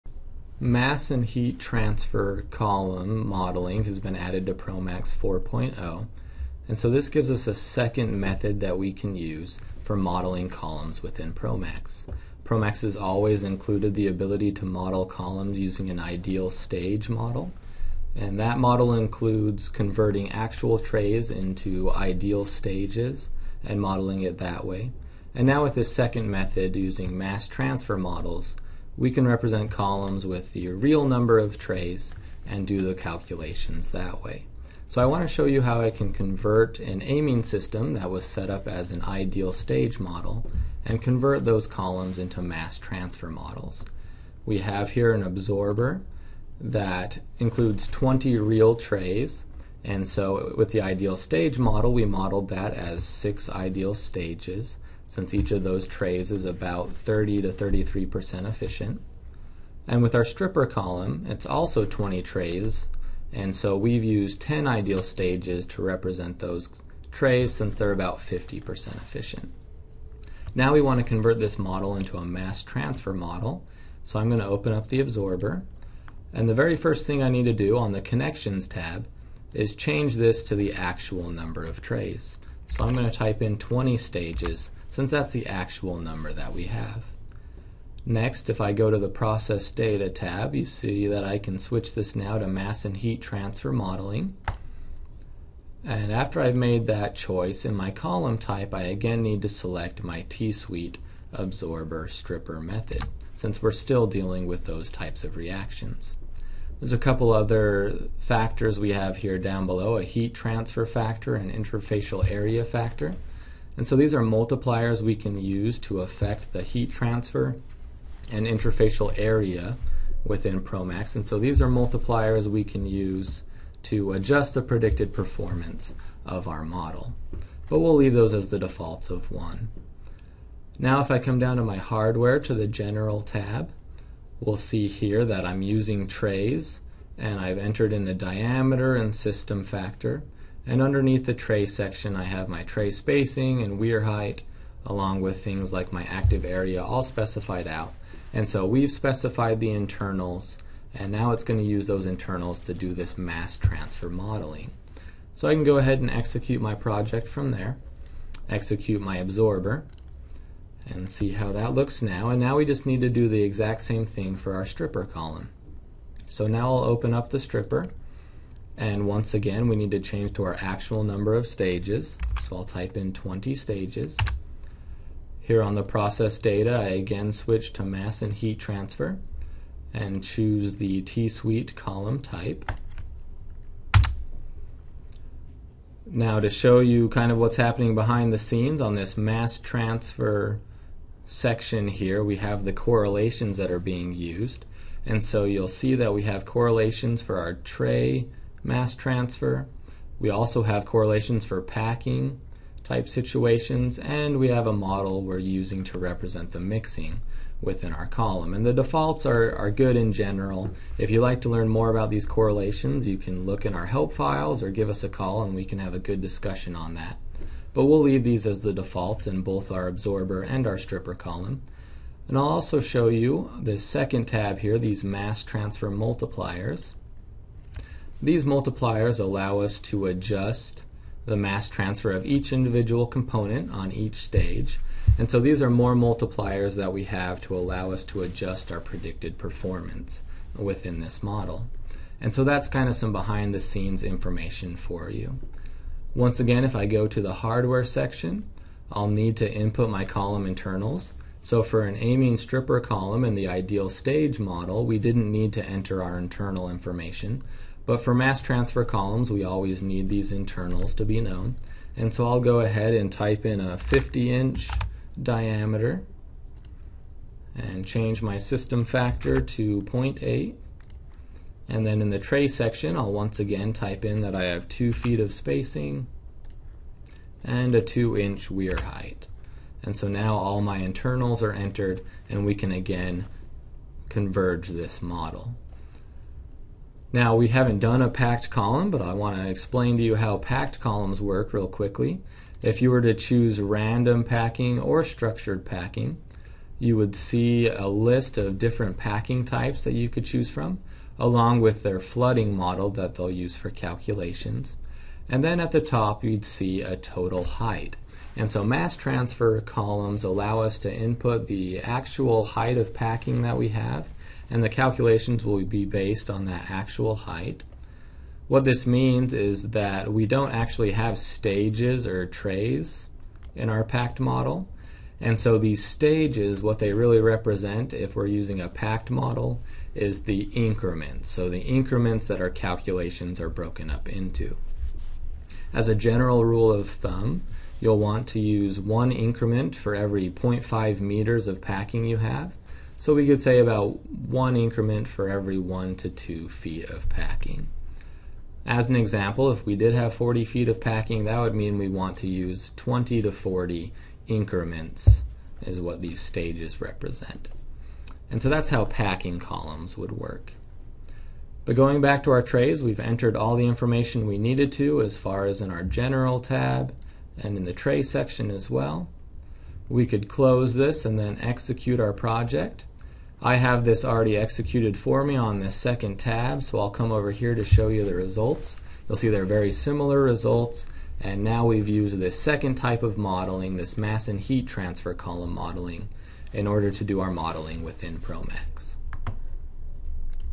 ProMax Mass Transfer Columns
This video demonstrates how to setup a mass transfer column within ProMax 4.0.